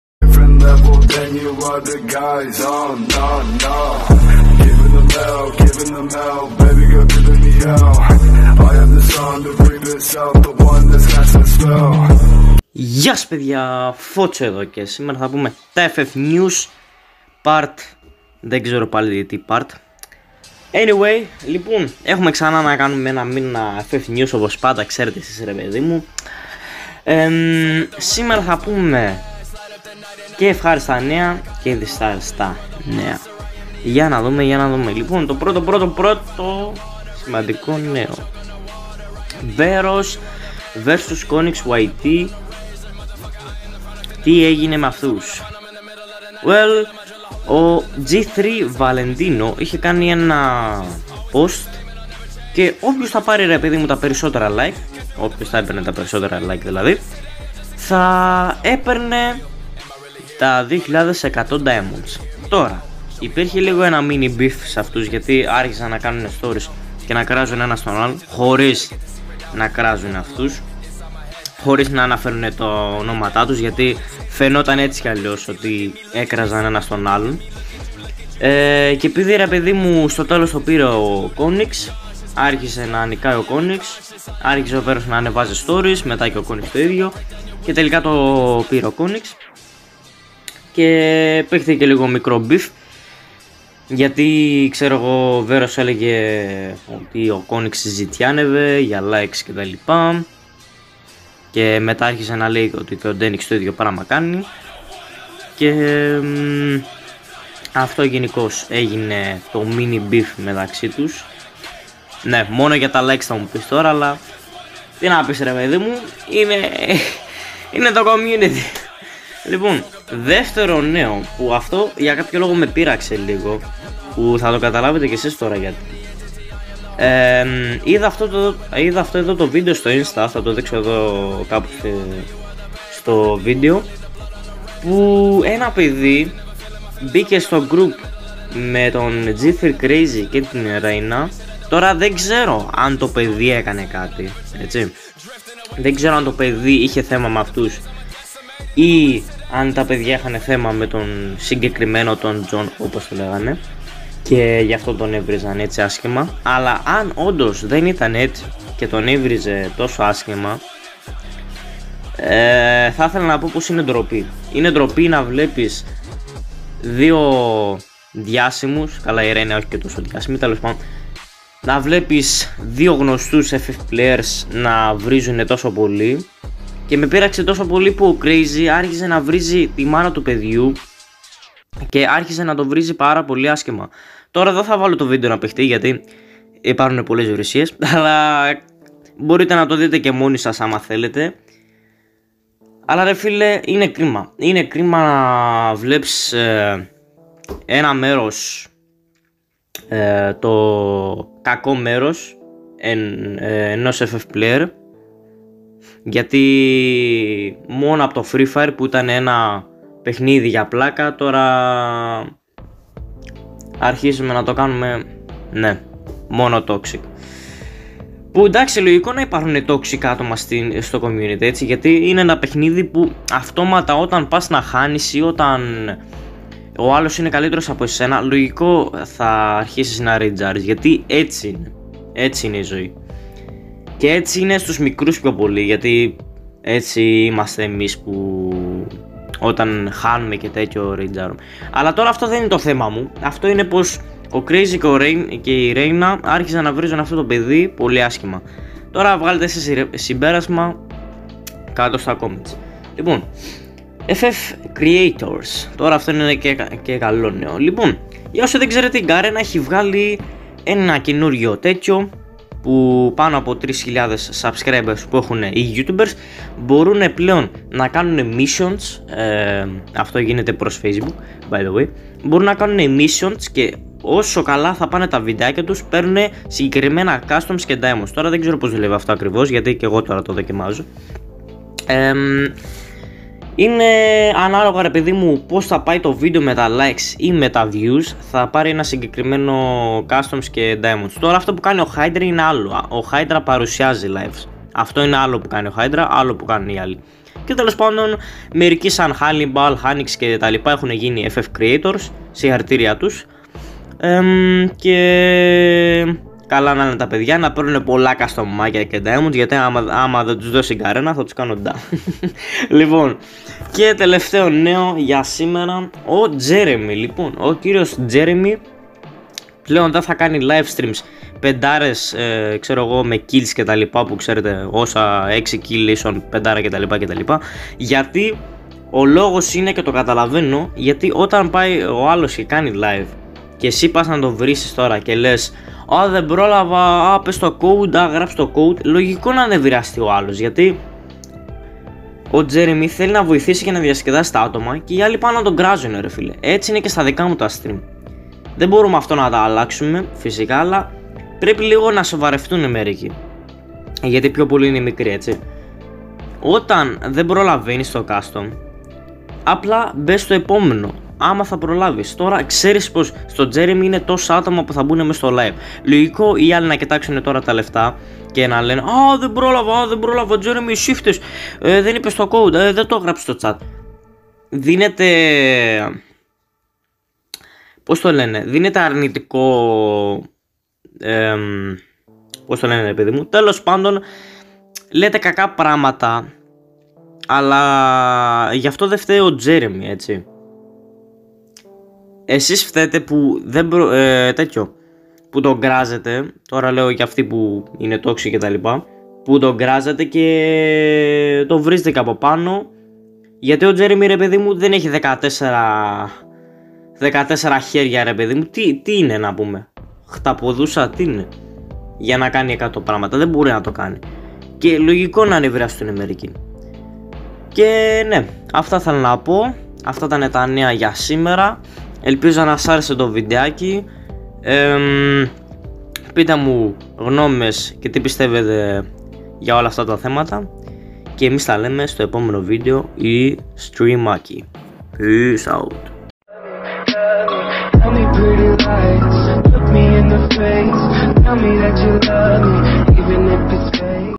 Jeremy τέλος giveaways?/verros ff vs konix yt/g3 crazy+Regina toxic?FF CREATORS[FF NEWS GR #23]
Γεια ελπίζω να σας αρεσε το FF NEWS GR Και θεκω να άκουσα την γνώμη σας

FF CREATORS📩

οι FF CREATORS περνούν customs και diamods ανάλογα με τα views Και likes που περνούν

BYE BYE ZEMRES😎👍❤